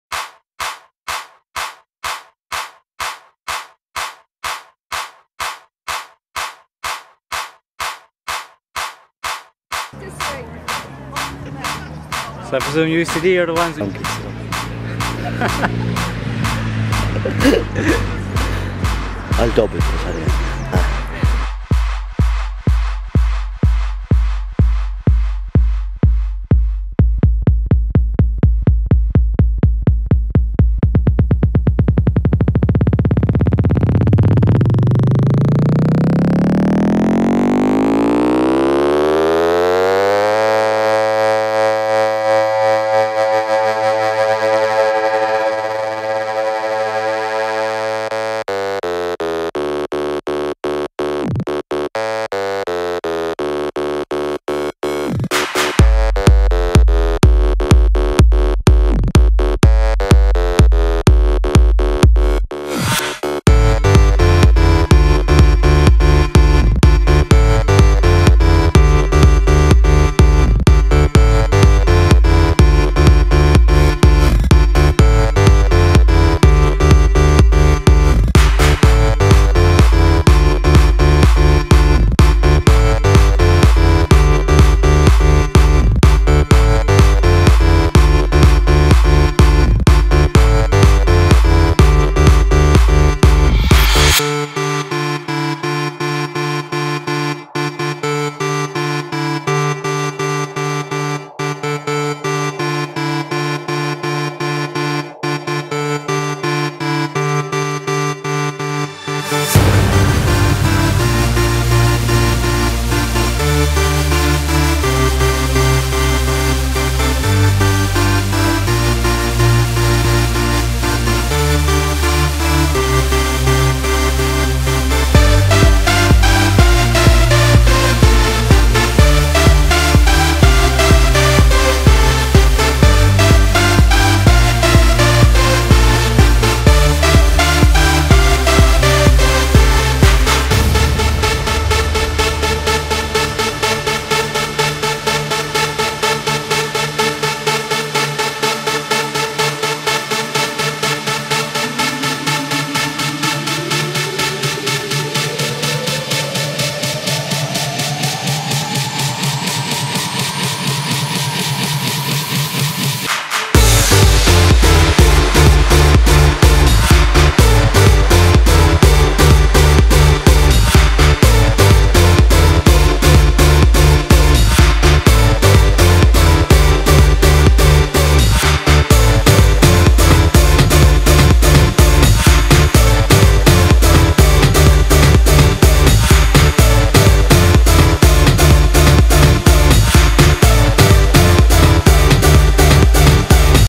Softball Intervarsities 2011
A Video of The Softball Intervarsities at the University of Limerick Sat.9th April 2011/